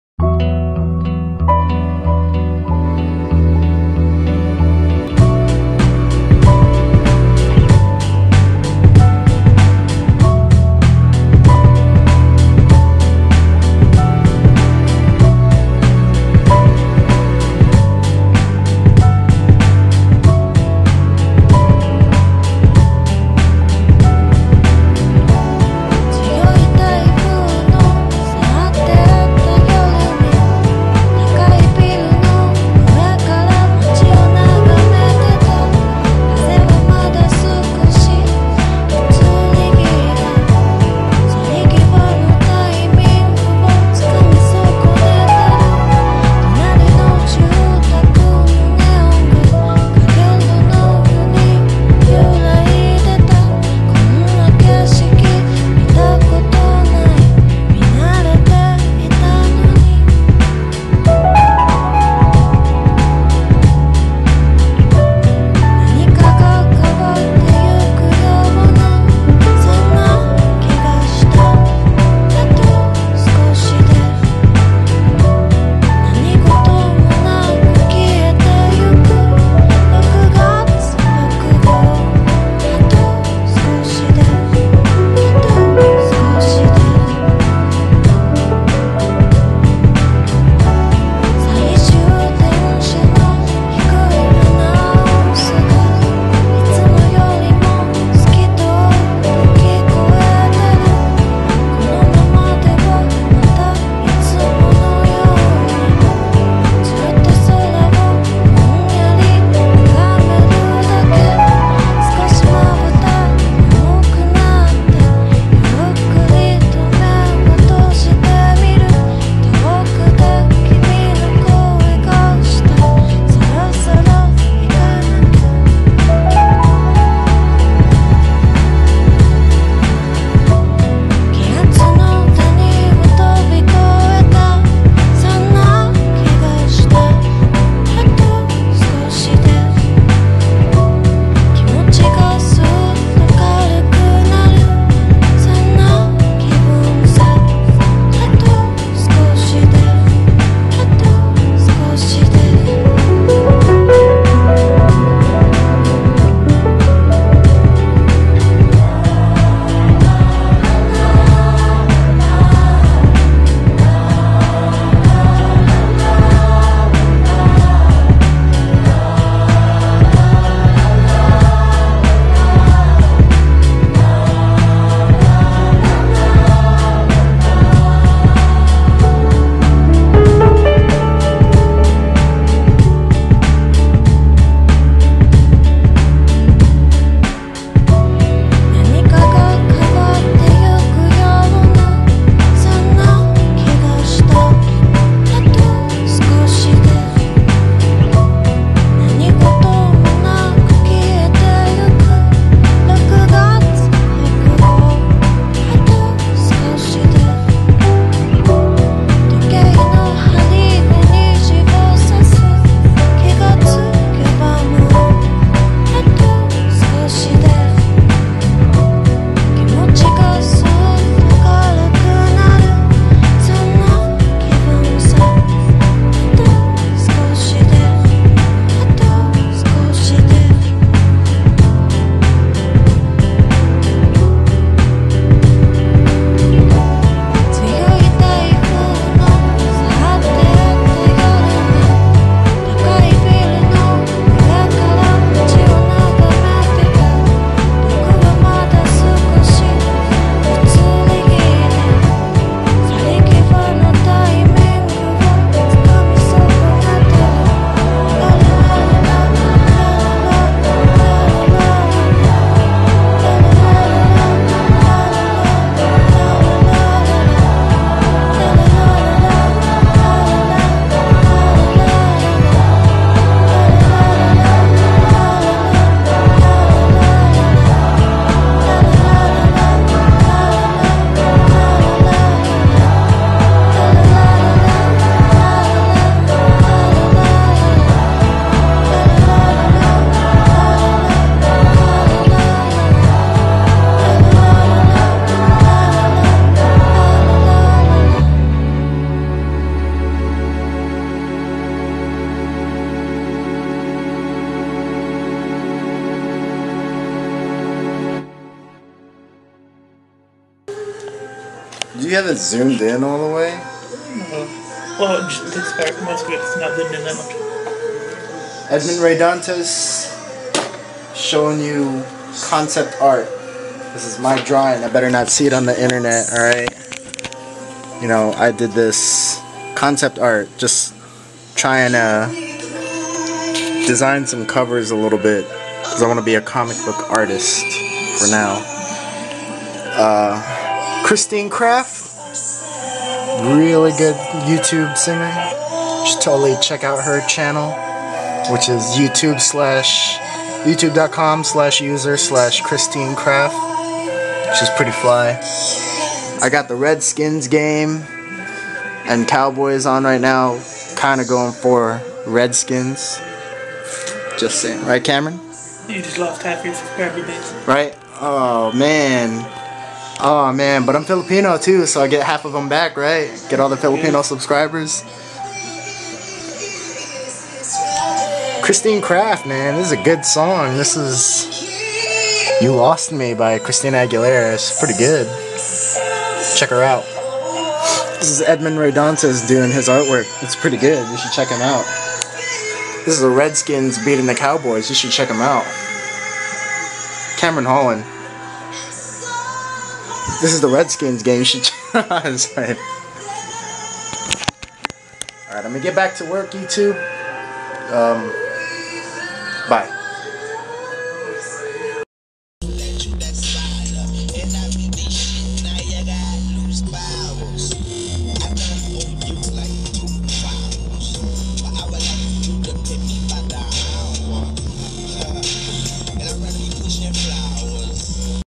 Me drawing concept art
I'm not trying to imitate anime or manga style. I respect the people who do anime and manga and understand that its really not that different from American comic book art but I DO NOT want to do anime/manga style.

Nujabes is good music. Thats why its playing.